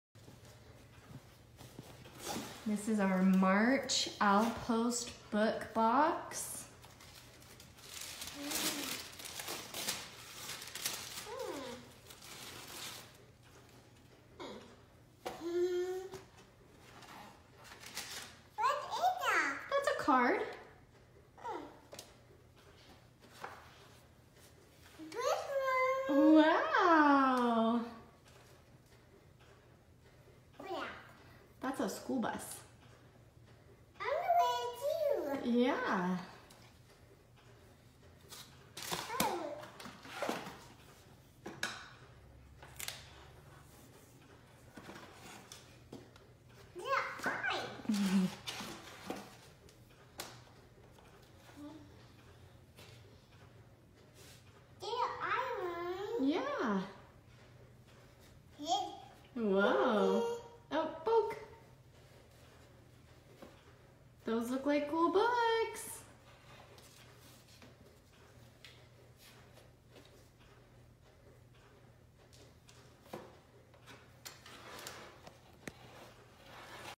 An oldie but a goodie! March Unbloxing!
Owl Post Books is a monthly subscription book subscription for kids and teenagers! Get 3 new books and a fun craft every month! There are subscriptions for ages 0-3, 4-7, 8-10, 11-13 and YA.